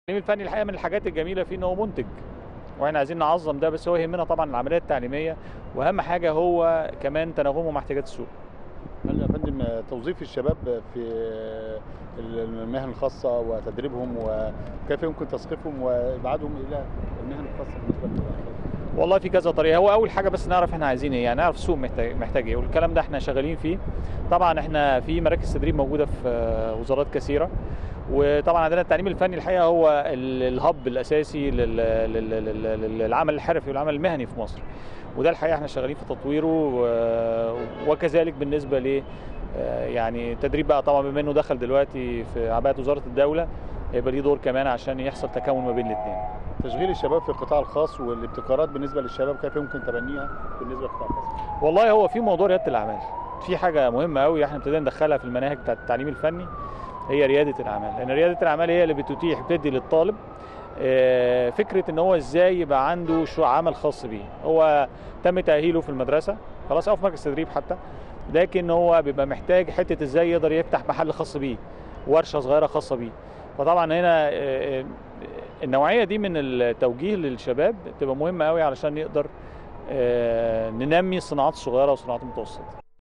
أخبار مصر: تصريحات د.محمد أحمد يوسف .. وزير التعليم الفني والتدريب الجديد
وزير التعليم الفني والتدريب: سأركز على إعداد خطة متكاملة للتدريب